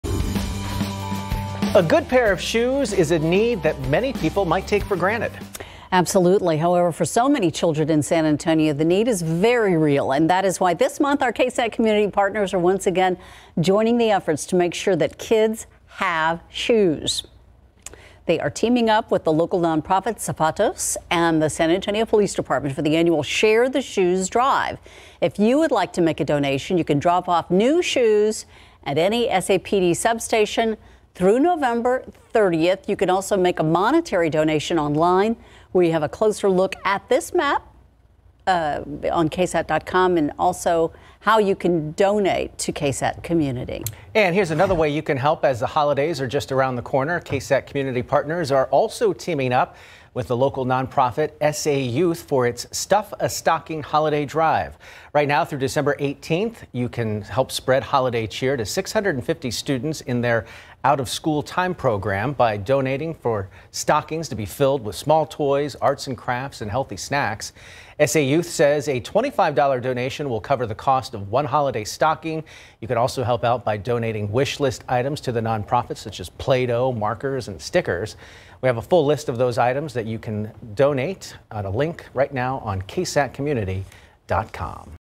KSAT Community: Help children in need this holiday season with the 'Stuff-A-Stocking' drive and ...
This November, KSAT Community is joining in the effort to make sure kids have proper footwear by teaming up with local nonprofit Zapatos, Inc. and the San Antonio Police Department for the annual KSAT Community “Share the Shoes” drive to collect new shoes of all sizes for toddlers and teenagers.